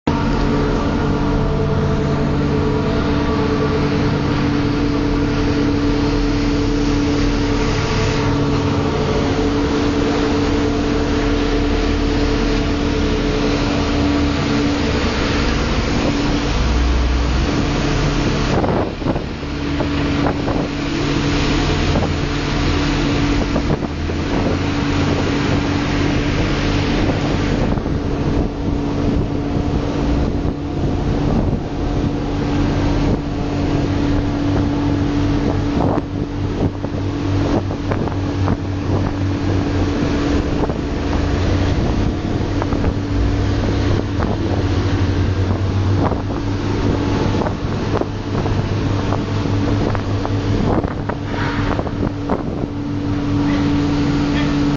Pilot boards Ferry whilst travelling at 15 knots brave man
Condor ferry approaching Cherbourg Harbour, France 4 th July 2009. Brave Pilot jumps aboard at 15 knots then Pilot boat gets stuck to the side of the ship until eventually manages to steer it to safety.